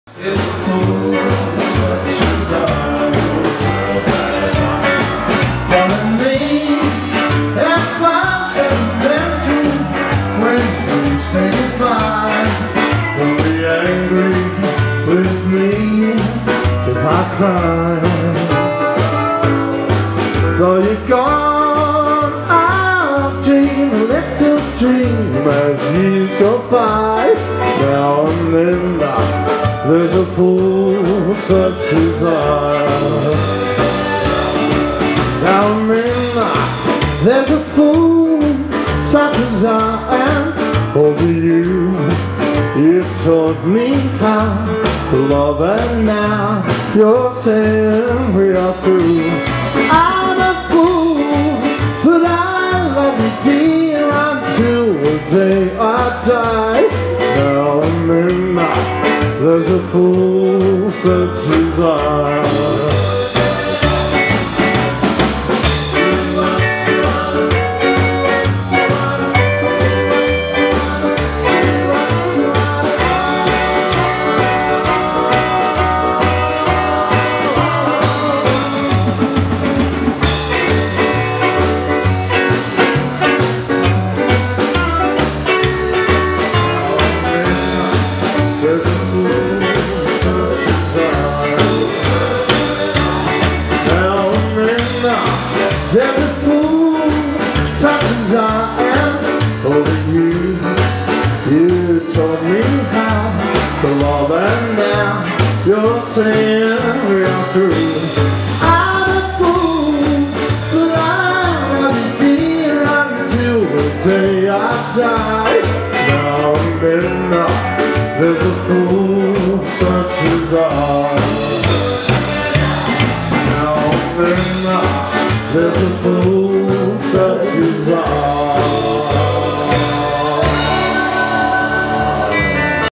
The flaming stars band at Ashfield RSL club NSW
Great band to dance to full of energy worth seeing at the Ashfield RSL club on a friday night at 8pm 374 Liverpool road Ashfield NSW  has a great dance floor and plenty dancer on the dance floor dancing rocknroll and rockabilly styles